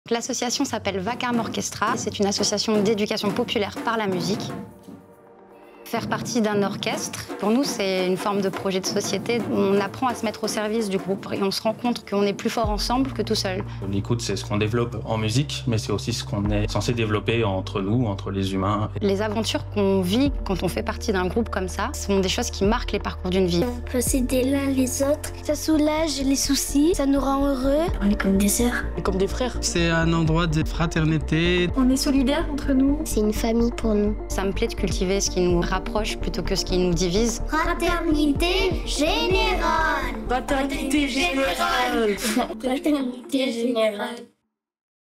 Vacarme Orchestra
Pour sa 8 ème saison, Fraternité Générale vous fait découvrir les récits inspirants des femmes et hommes qui s’engagent dans notre campagne France Fraternelle - épisode 12/14 🎬

L'association Vacarme Orchestra a installé ses tambours au centre-ville de Marseille il y a trois ans, entre la gare saint-charles et le vieux port. Depuis, une centaine d'habitant.e.s du quartier vit au rythme de la batucada*, répète chaque semaine, se produit en spectacle, anime des fêtes et défilés et mène des projets collectifs épanouissants.

Vacarme Orchestra s'inscrit dans une démarche d'éducation populaire de la jeunesse et promeut les droits culturels pour toutes et pour tous. Elle favorise l'accès à la pratique musicale et à ses nombreux bénéfices. La dimension collective de la pratique offre des expériences variées et un apprentissage de la citoyenneté émancipateur qui renforce la solidarité, la dignité et la confiance en soi. L'association contribue à la dynamique d'animation culturelle du territoire par ses habitant.e.s, en mettant l'accent sur la parole et la place des femmes et des enfants dans l'espace public.

Apprendre et pratiquer la musique, se passionner, se connaître, être membre d'un groupe, organiser et jouer des spectacles, vivre des expériences hors du commun, faire de nouvelles rencontres et étendre ses horizons, voilà un aperçu du programme que chacun.e d'entre nous devrions pouvoir suivre un jour dans sa vie. C'est l'aventure inédite dans laquelle s'embarquent les familles membres du Vacarme Orchestra, tambours battants.

* un ensemble de percussions brésilien répandu dans le monde.

Avec France Télévisions et le CNC.